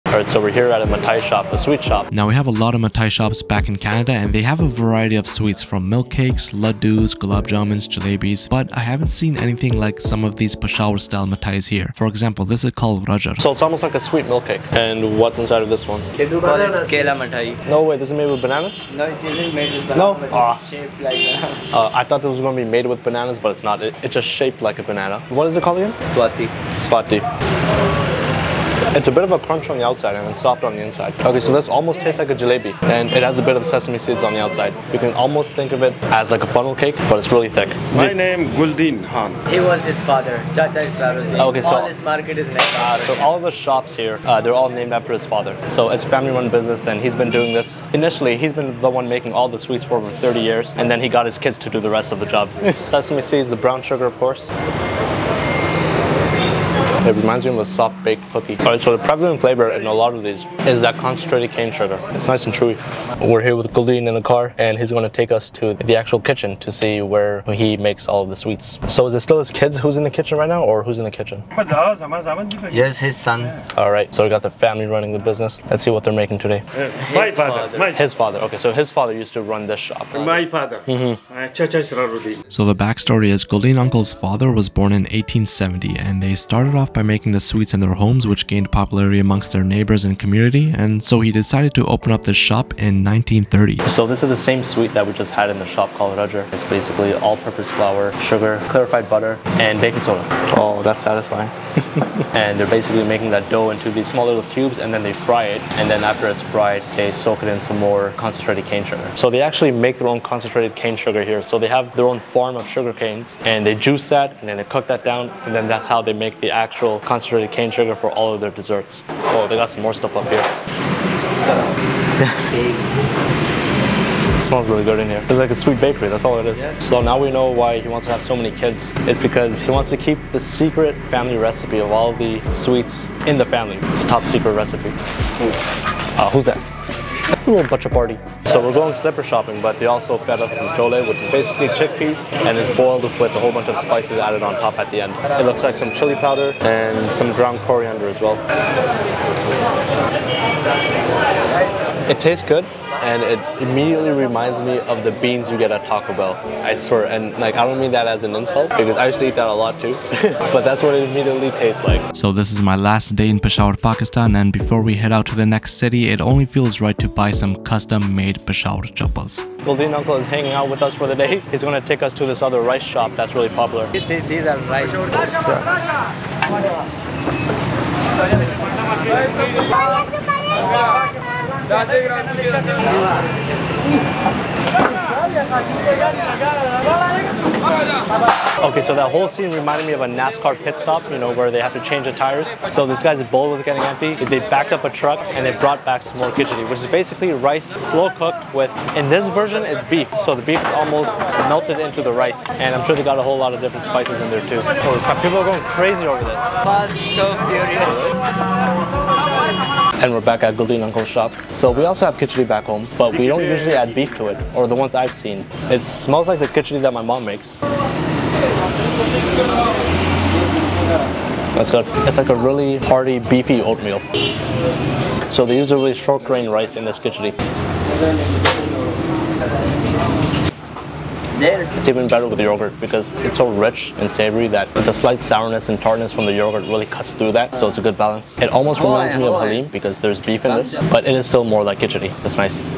Is this Pakistan's OLDEST Sweets Shop?
Dang this shop existed before Pakistan even became a country.

My parents immigrated to Canada from Pakistan in the 80s. I was born and raised in Toronto where my mother fed me Pakistani dishes each and every day. I always wondered, how good is the cuisine from Pakistan in comparison to what I grew up eating?

I can't wait to share this trip with ya'll. More episodes of each city will be uploaded soon.

0:00 Mithai (Sweets)
1:03 Dessert Factory
2:18 Cholay (Chickpeas)
2:50 Khichdi (Rice and Lentils)

Much love to Falcon Adventure for assisting me on this epic food journey!

Audio: Musicality